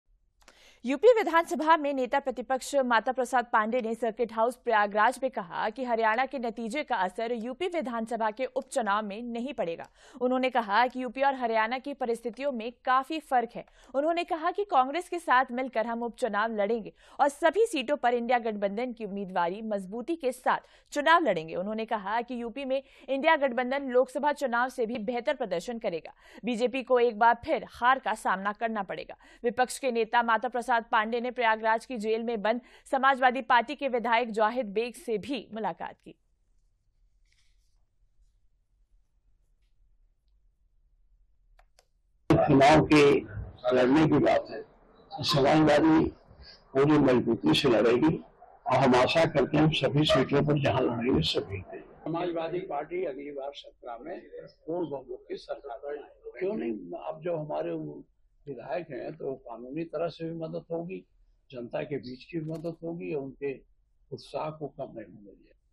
हरियाणा के नतीजे का असर यूपी विधानसभा के उप चुनाव में नहीं पड़ेगा : नेता प्रतिपक्ष माता प्रसाद पांडे
यूपी विधानसभा में नेता प्रतिपक्ष माता प्रसाद पांडे ने सर्किट हाउस प्रयागराज में कहा कि हरियाणा के नतीजे का असर यूपी विधानसभा के उप चुनाव में नहीं पड़ेगा उन्होंने कहा कि यूपी और हरियाणा की परिस्थितियों में काफी फर्क है।  उन्होंने कहा कि कांग्रेस के साथ मिलकर हम उपचुनाव लड़ेंगे सभी सीटों पर इंडिया गठबंधन की उम्मीदवार मजबूती के साथ चुनाव लड़ेंगे उन्होंने कहा कि यूपी में इंडिया गठबंधन लोकसभा चुनाव से भी बेहतर प्रदर्शन करेगा।  बीजेपी को एक बार फिर हार का सामना करना पड़ेगा।  विपक्ष के नेता माता प्रसाद पांडे ने प्रयागराज की जेल में बंद समाजवादी पार्टी के विधायक जाहिद बैग से भी मुलाकात की।